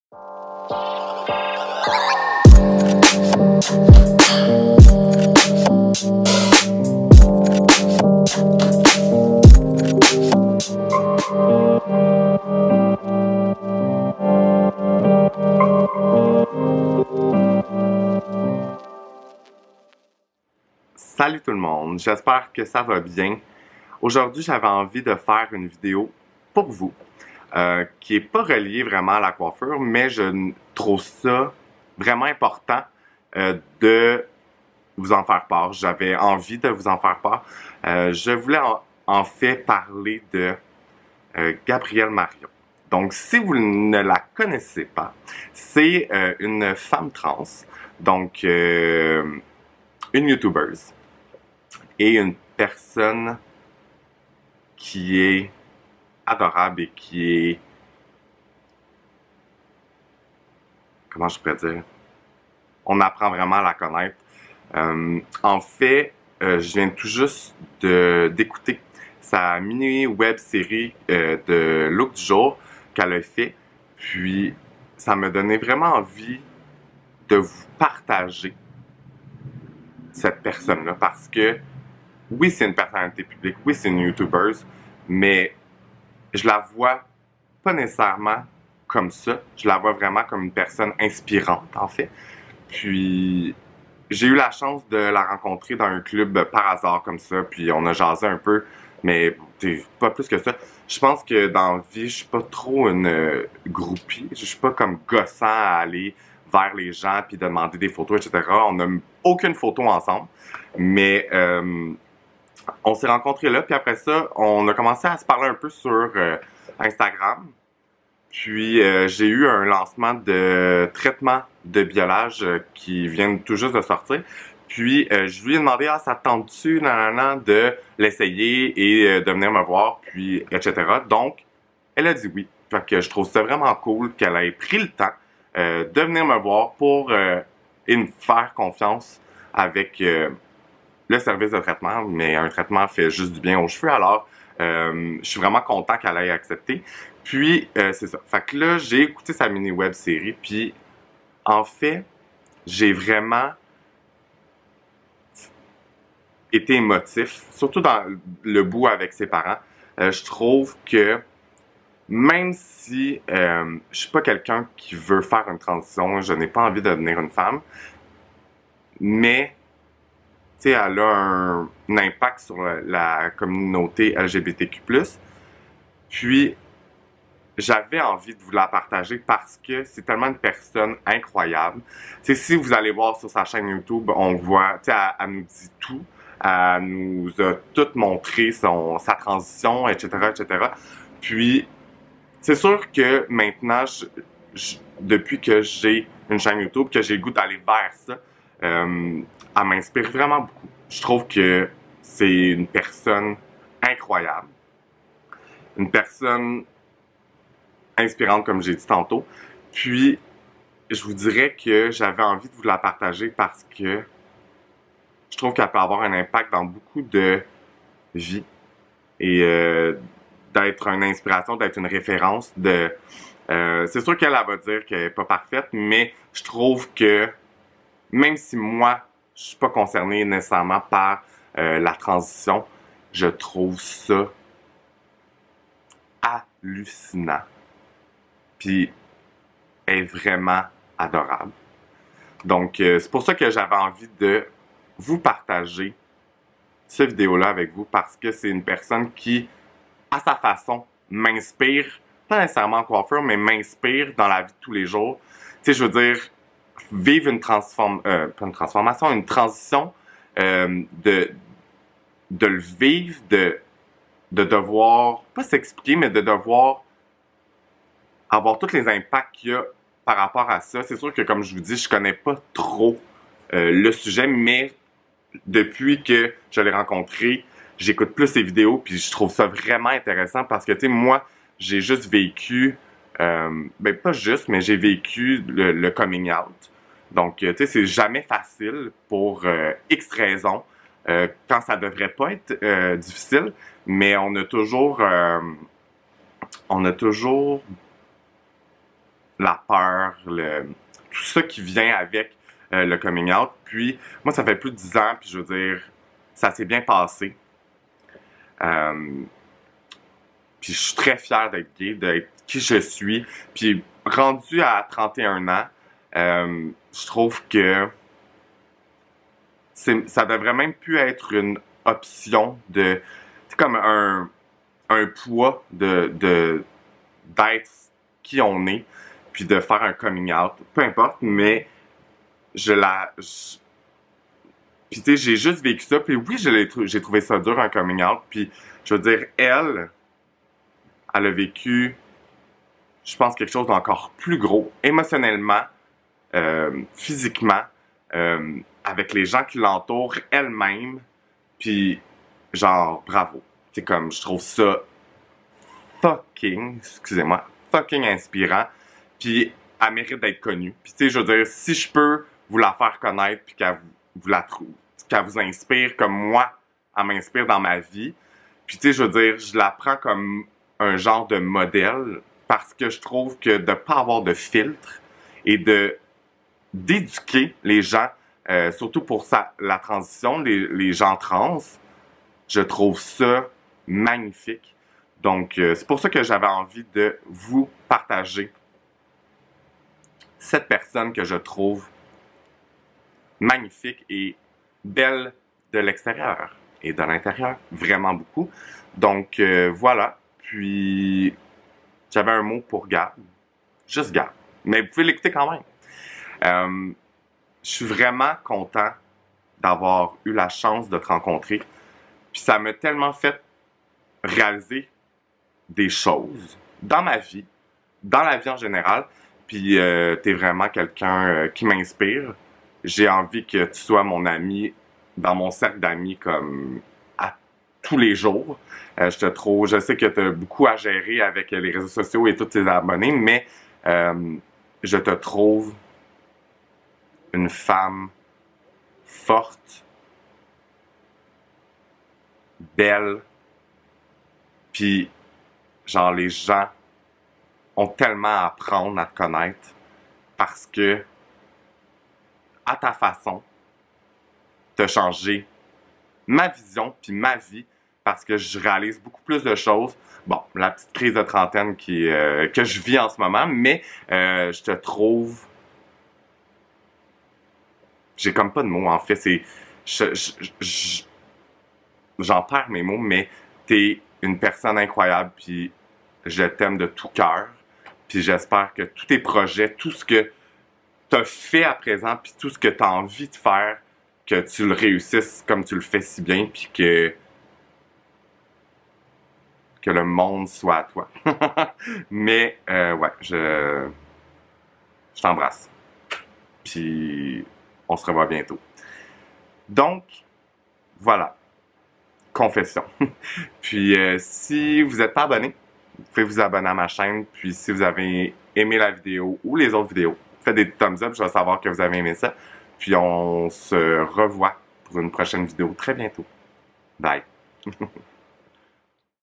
GABRIELLE MARION
J'AVAIS ENVIE DE VOUS FAIRE DECOUVRIR UNE NOUVELLE RENCONTRE QUE J'AI FAIT CETTE ANNÉE ET À SA FACON ELLE M'INSPIRE DANS MA VIE DE TOUT LES JOURS !
JE T'AIME GAB !XX